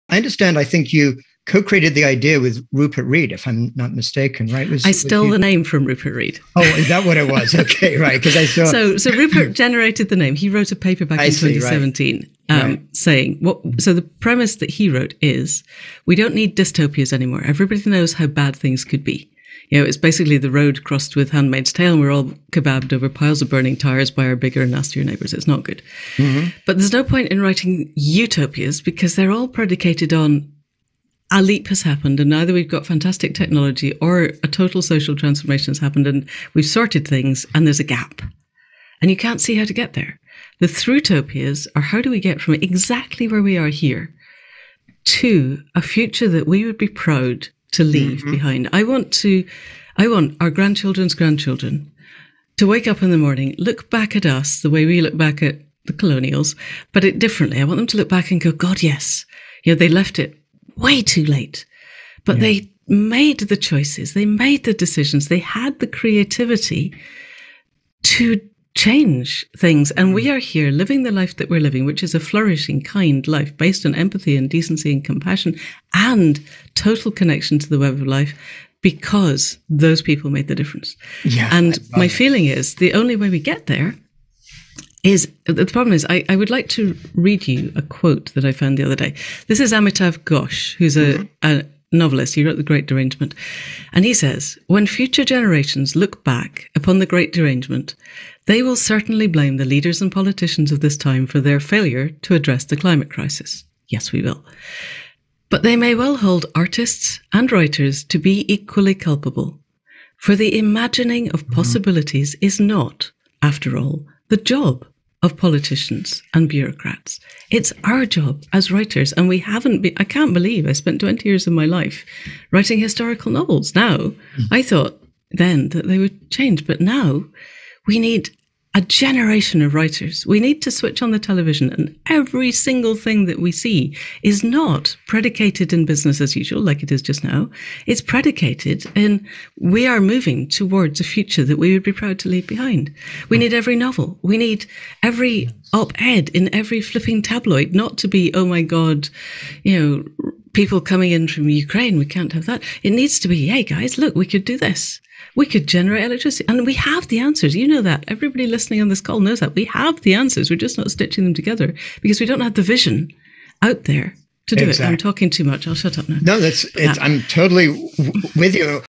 Manda Scott: "We don't have the vision out there"
Engaging routes through to a world we’d all be proud to bequeath to future generations...

This interview with Manda Scott, conducted by Jeremy Lent, is an excerpt from Deep Transformation's Live Network Meeting on 3 May 2022.

Manda Scott is a novelist, currently writing a Thrutopian novel, organiser of the Thrutopia Masterclass, smallholder, shamanic dreamer, renegade economist and podcaster.

Jeremy Lent is an author whose work investigates the underlying causes of our civilisation’s existential crisis and explores pathways toward a life-affirming future.

Deep Transformation Network is a global community to explore pathways to an ecological civilisation.

"We know that inspiring stories shape our futures. Throughout human history, our power to imagine a better world has pushed – or pulled – us away from the comfort of the known. But our stories of power, accumulation and self-aggrandisement have brought us to the edge of extinction and we urgently need new heroes, new ways of doing things, new peaks to aim for."
~ Manda Scott